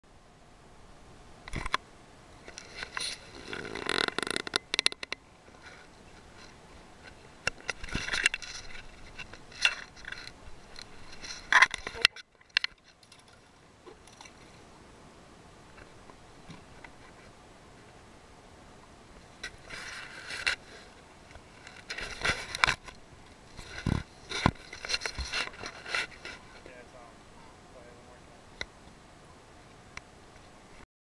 nice attempt at taking an image with my digital camera dad....lol
16 and 3/4 smallie from the Bourbeuse River. Fathers Day paddle